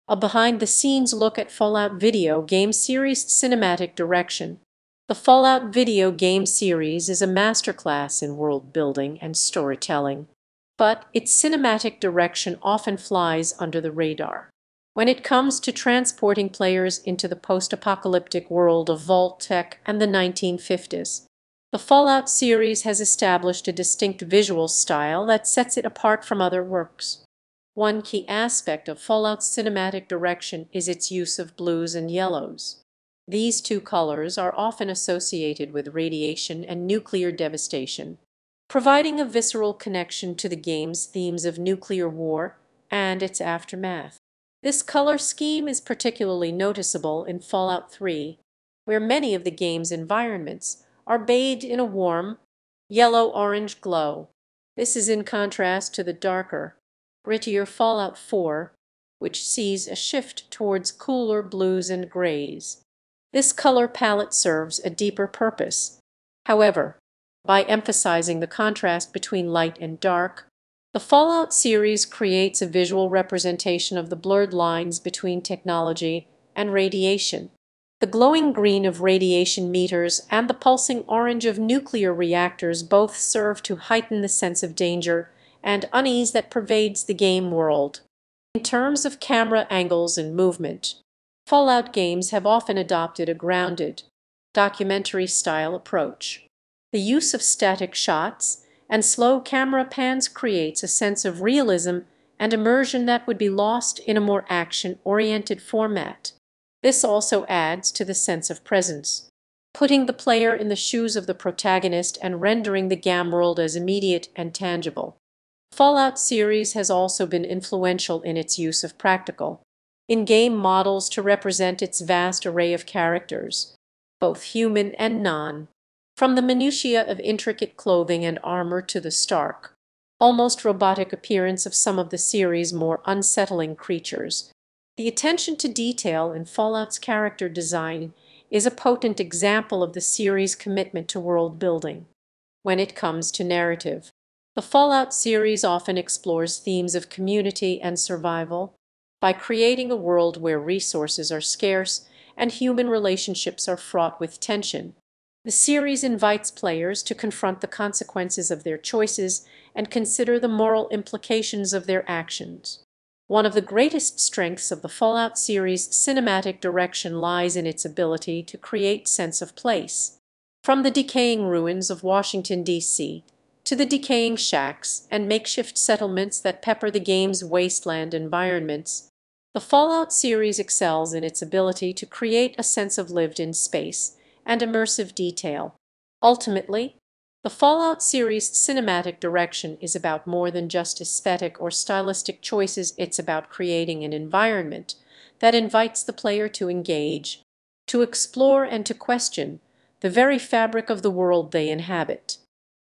A Behind-the-Scenes Look at Fallout Video Game Series' Cinematic Direction
This video contains material generated with artificial intelligence, and may contain errors.